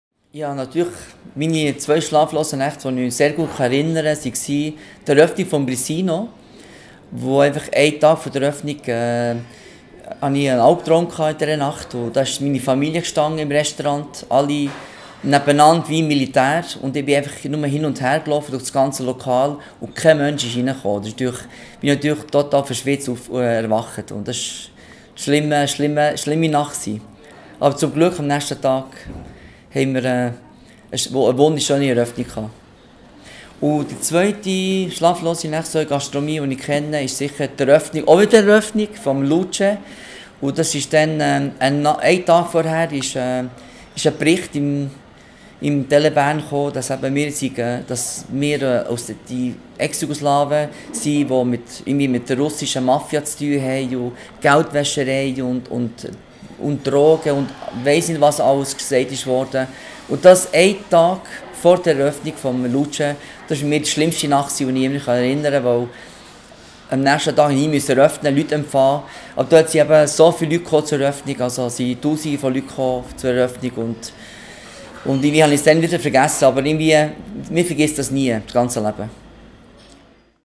Stevos Nilovics schlimme Nächte
In den 36 Jahren als Gastronome gab es für Luce-Wirt Stevo Nilovic zwei schlaflose Nächte.